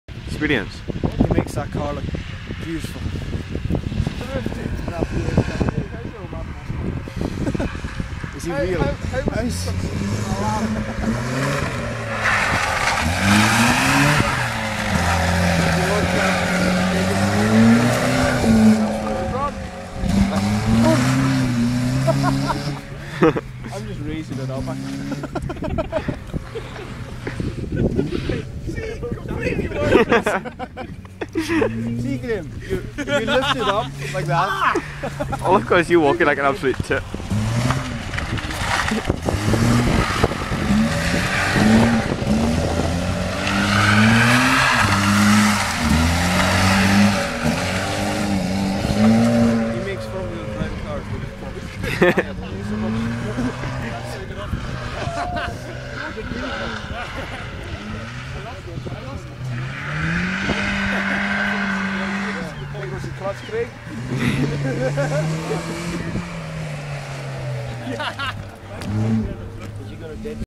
Lewis Car Club Autotest: Fiesta
December autotest
My dad in our fiesta!